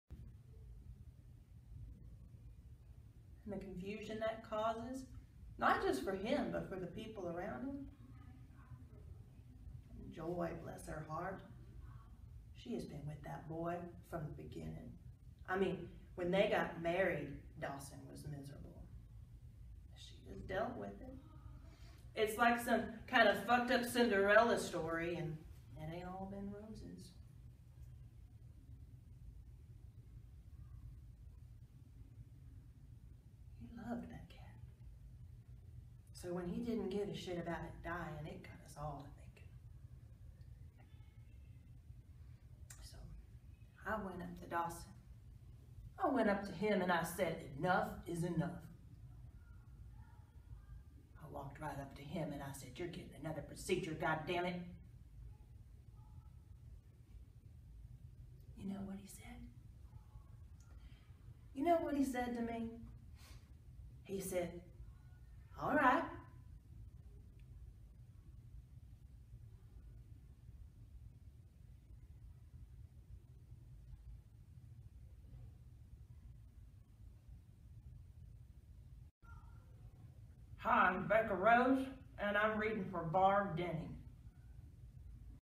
Becka Rose Barb Dennings
Becka Rose captures the great Barb Dennings by the butt...cigarette butt, that is!